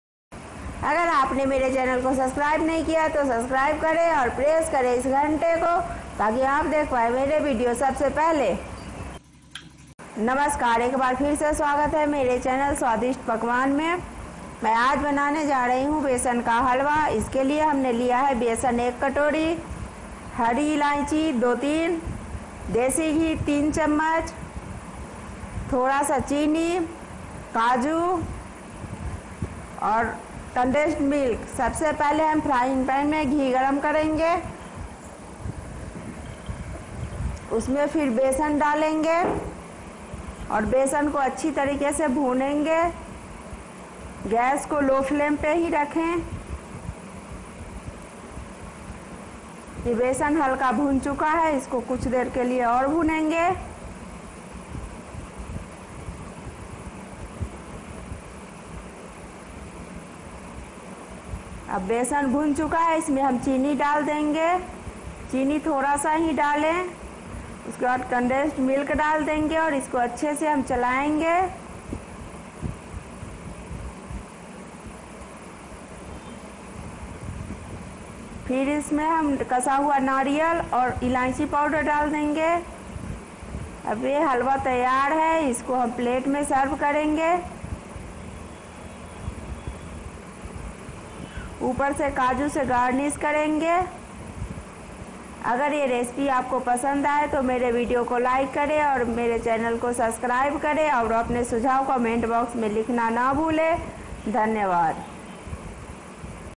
Besan ka halwa banane ki vidhi | Besan ka halwa recipe | Swadisht Pakwan
BEST AND CHEAPEST UTENSILS
Camera prefered
Utensils used
Kitchen clothes used

INGREDIENTS USED
Besan
Sugar
Elichi powder
Nariyal
Kaju
Condensed milk
Ghee